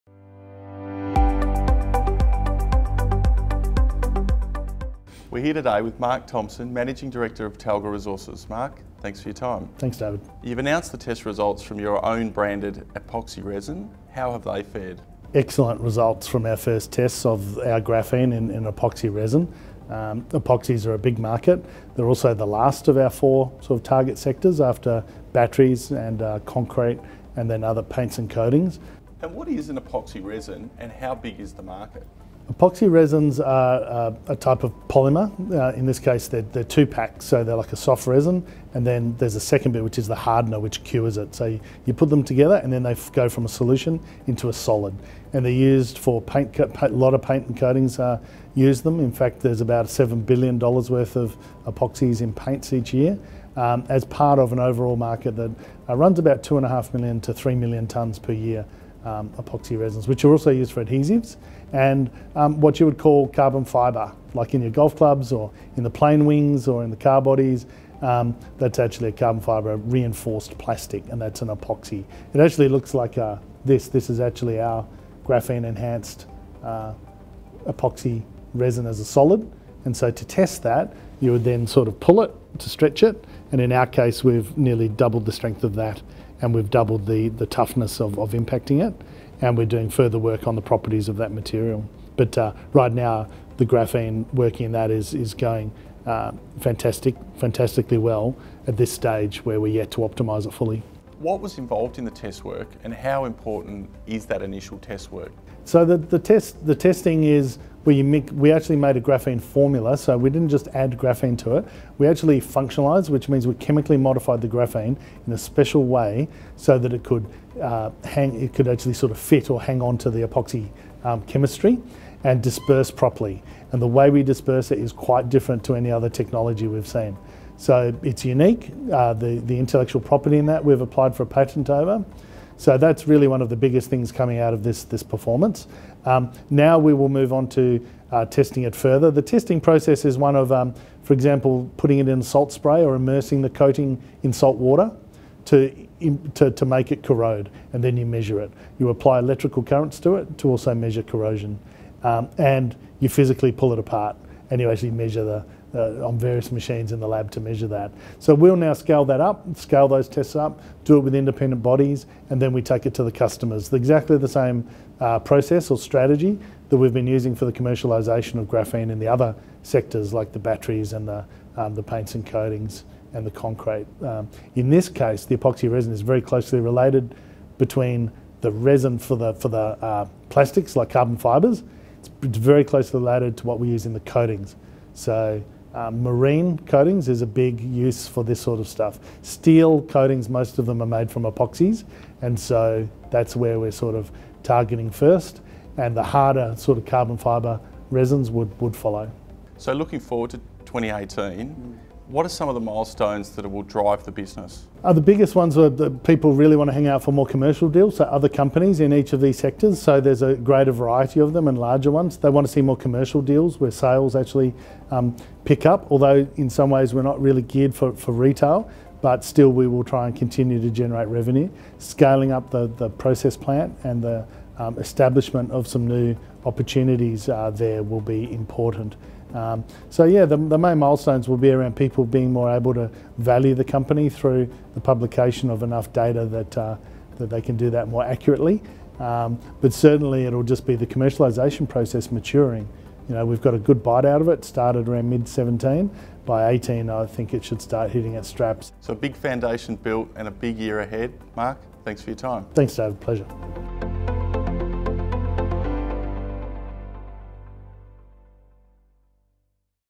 Managing Director Interview on Graphene Epoxy Release
Mark Thompson, Managing Director of Talga Resources Ltd (ASX: TLG), speaking to The Pick's David Tasker about the recent graphene epoxy release.